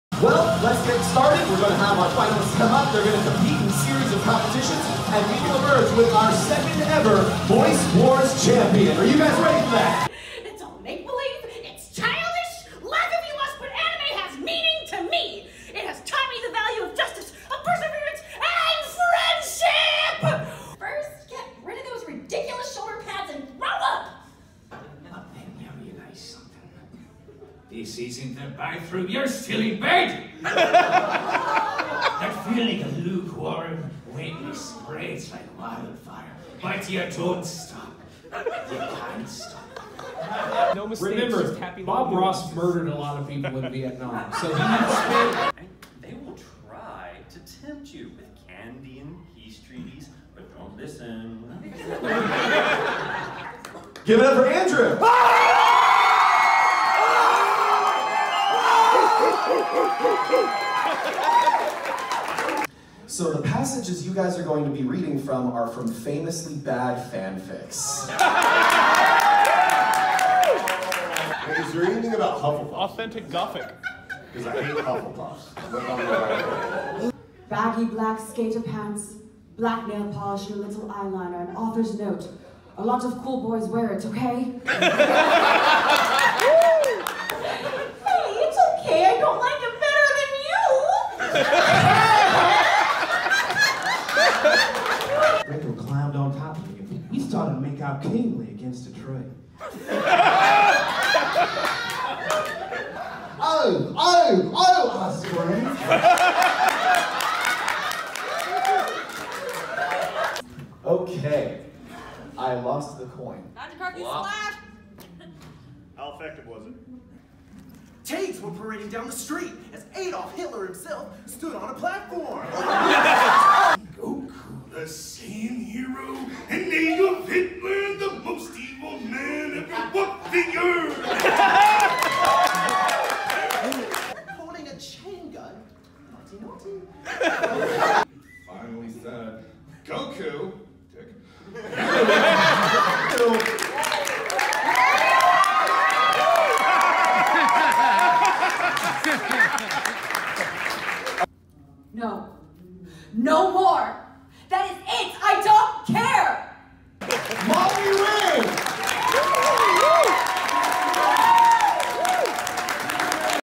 Voice Wars Anime Los Angeles 2020 Compilation
A short compilation of some of the best moments of Voice Wars at ALA 2020!

If you would like to bring this event to your local con or see it return to ALA, please recommend the panel!!